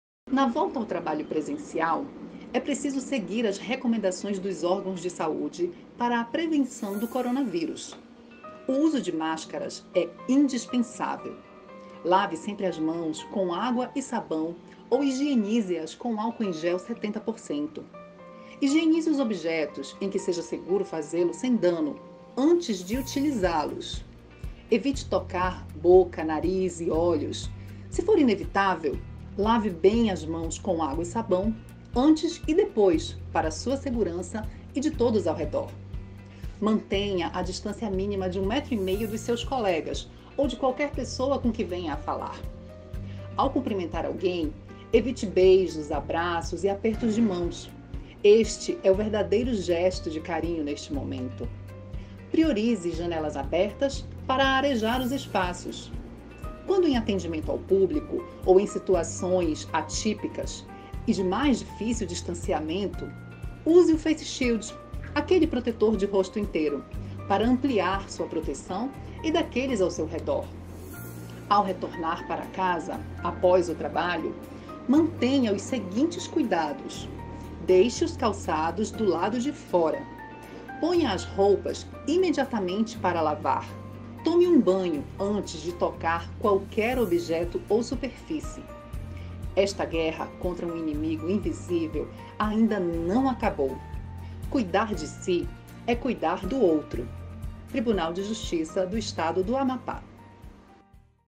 TJAP na prevenção à Covid-19
O Tribunal de Justiça do  Amapá se prepara para o retorno gradativo às atividades presenciais. E para preservar a vida de magistrados, servidores, colaboradores, jurisdicionados e demais membros do Sistema de Justiça, todos os cuidados devem ser tomados. Saiba mais: